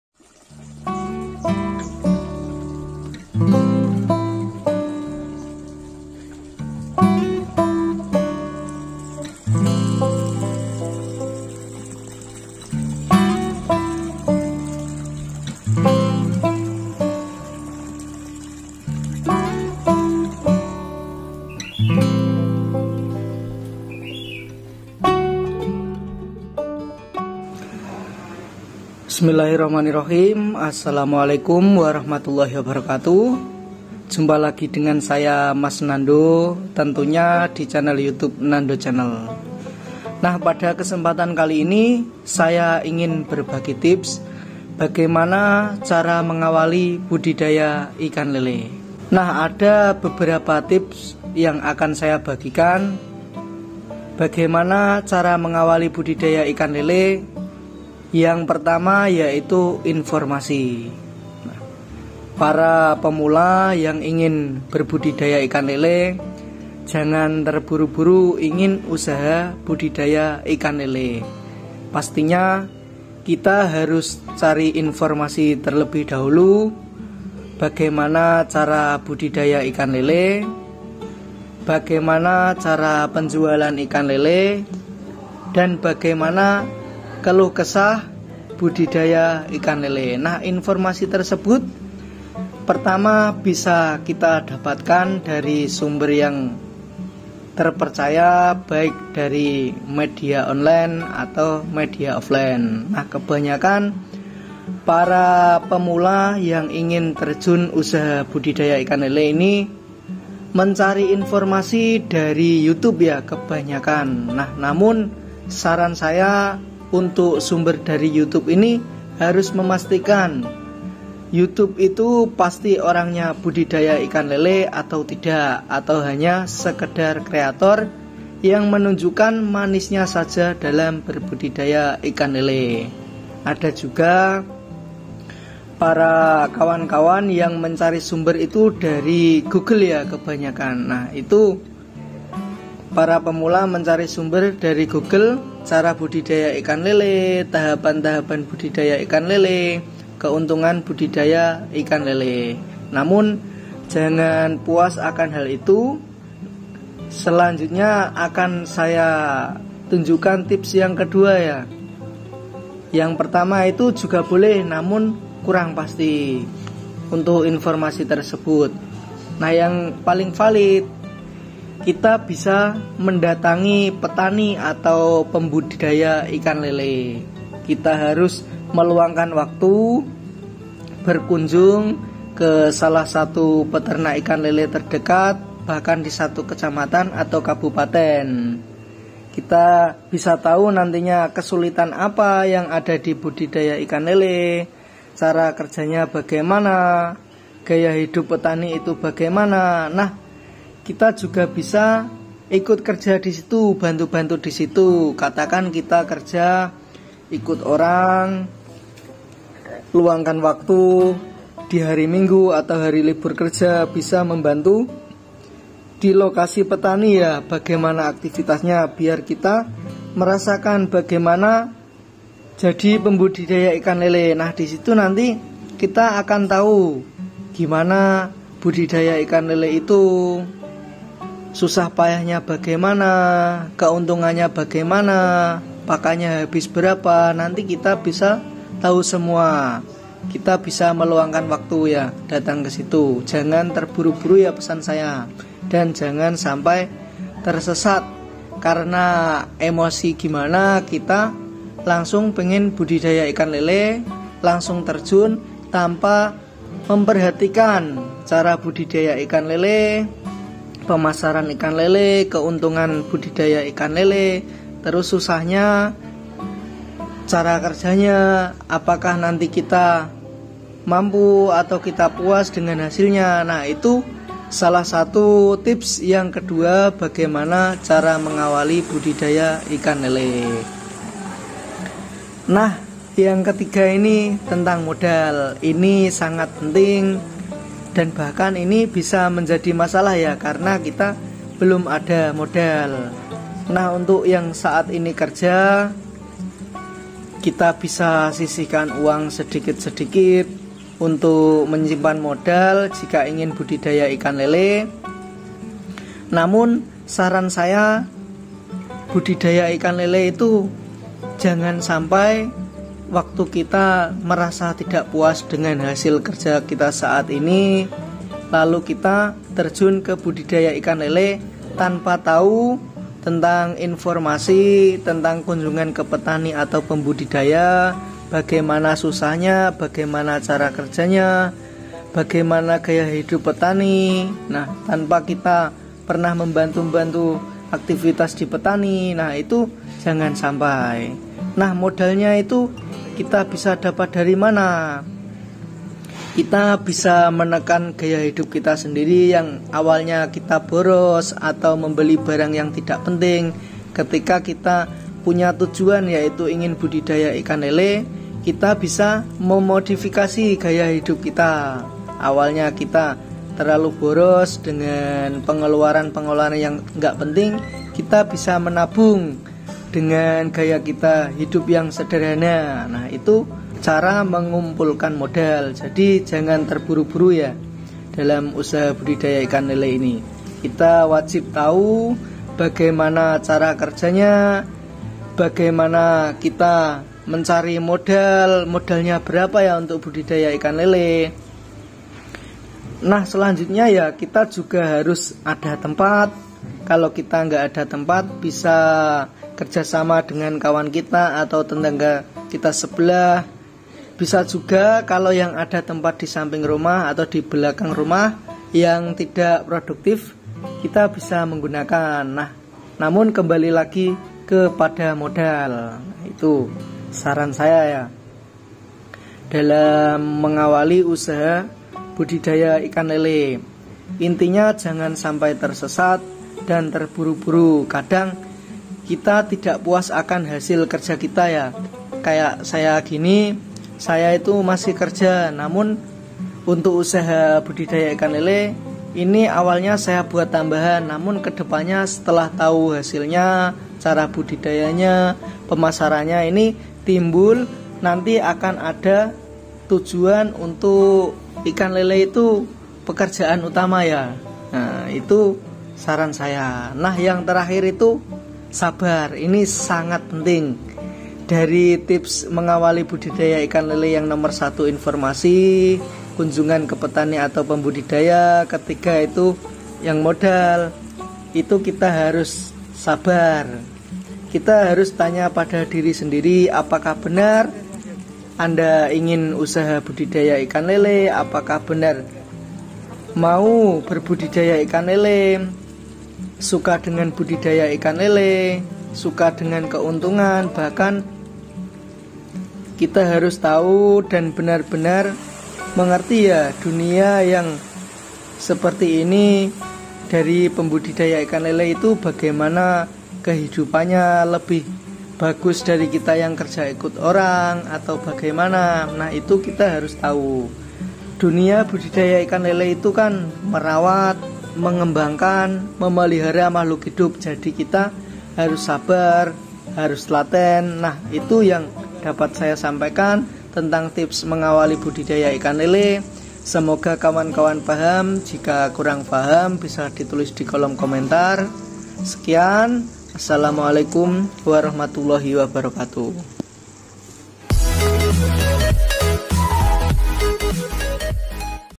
CARA MENGAWALI USAHA BUDIDAYA LELE
Bisnis Ternak Lele Sukses : Langkah langkah penting yang harus di ketahui untuk memulai ternak lele